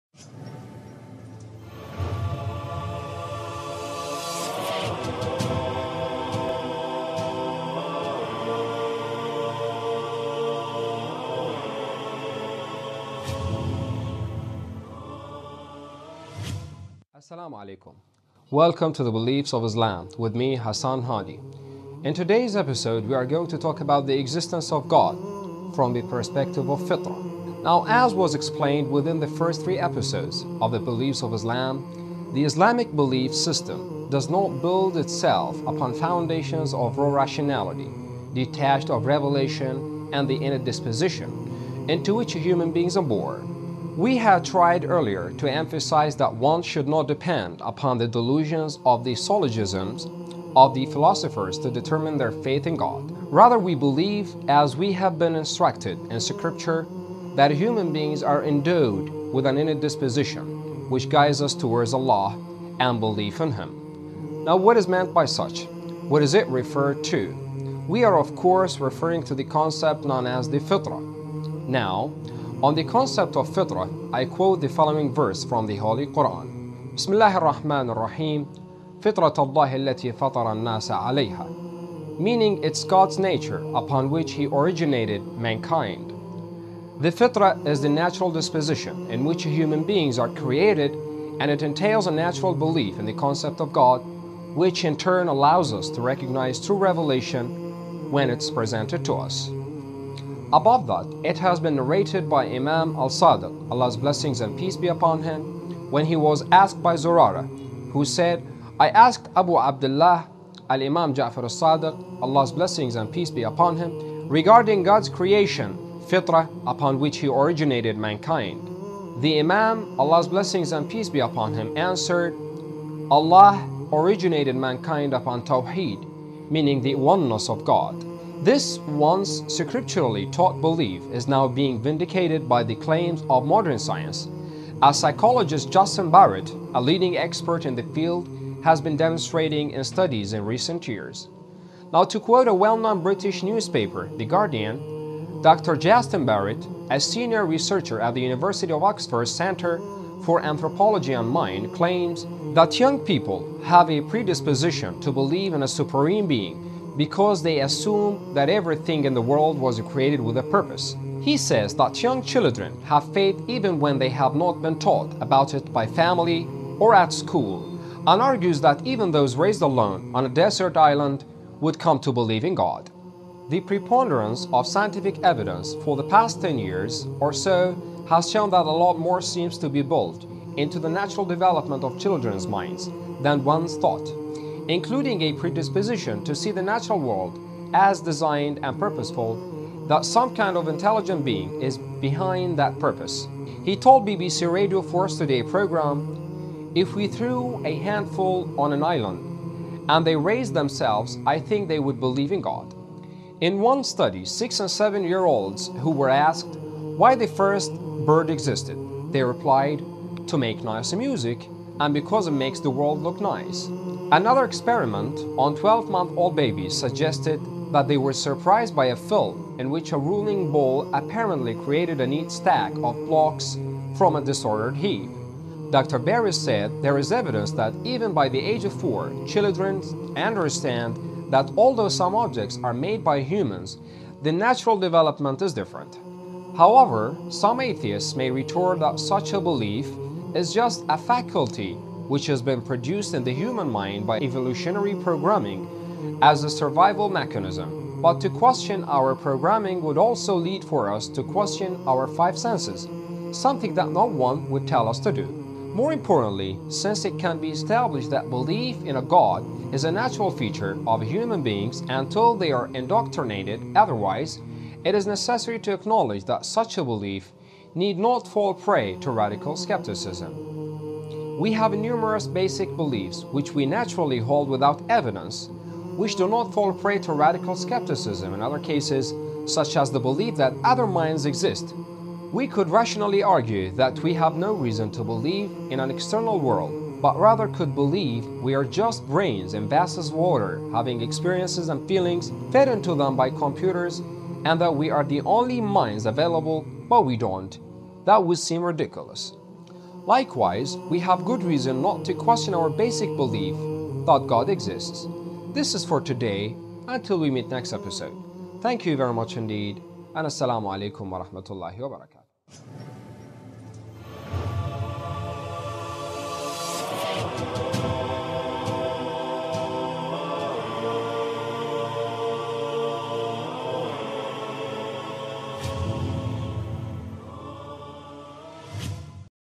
Islamic Beliefs | 10 | Hassan Hadi
Join us with these series of Islamic Beliefs by Hassan Hadi to know more about the rules of Islam and what does Islam recommended us to believe .